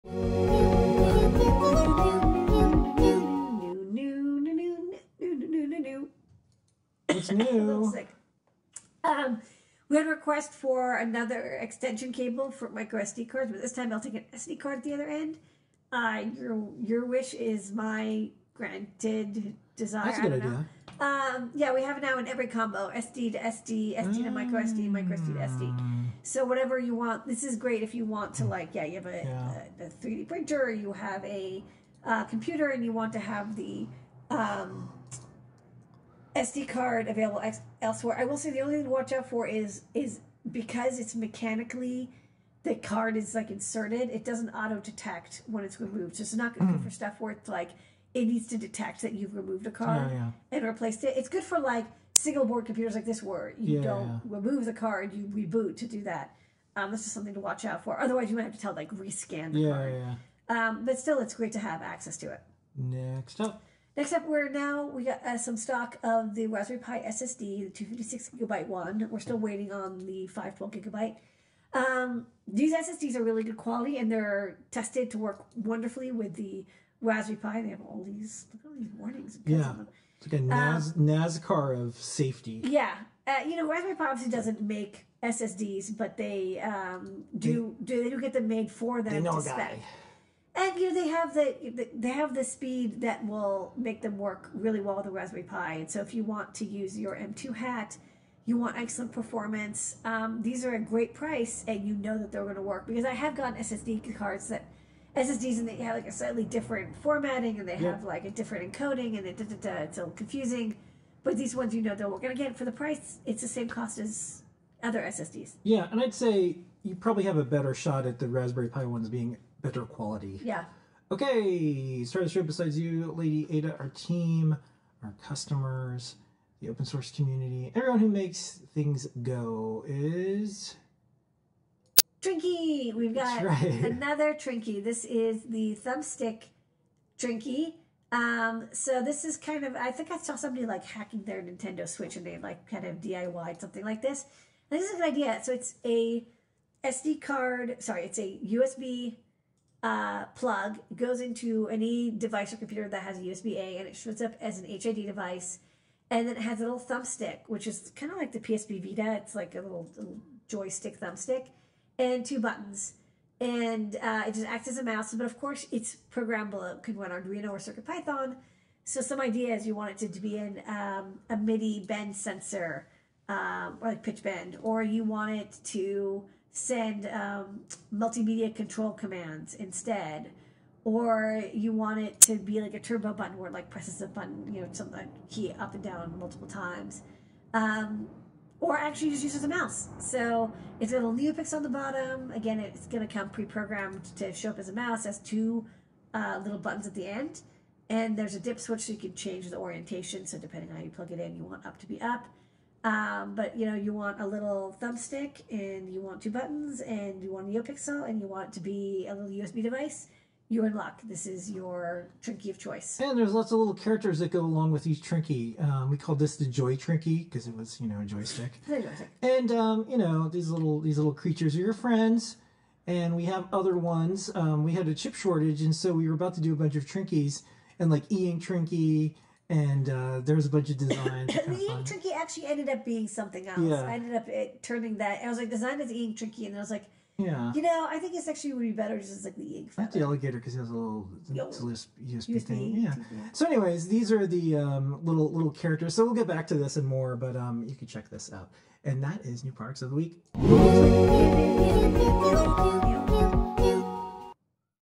NewProducts 11/20/24 Feat @Adafruit Thumbstick Trinkey - USB Key with Miniature Joystick
SD to MicroSD Extender - 23cm (9 inch) long cable (0:09)

Adafruit Thumbstick Trinkey - USB Key with miniature joystick (2:12)